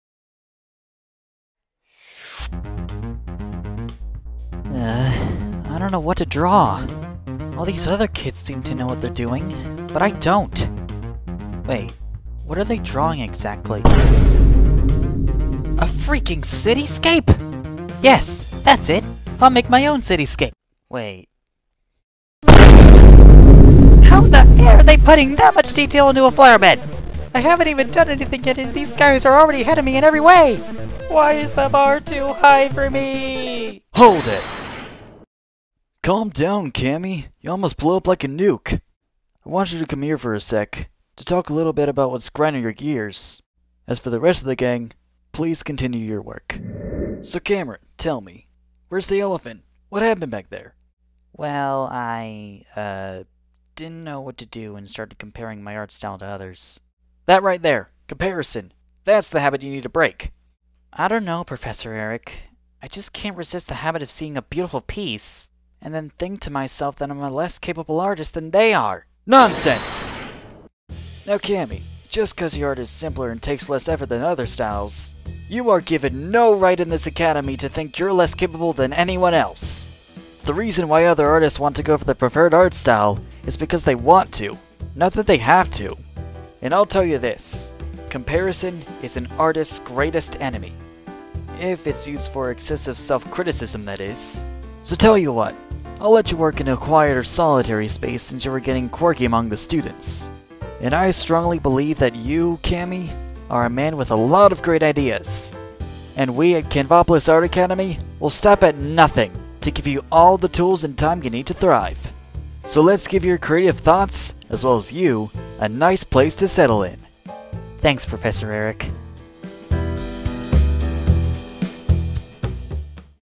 Comparison: an artist’s worst enemy | Camtoon
Prof. Erik wants to see no student of his fall short of inspiration, especially Cameron!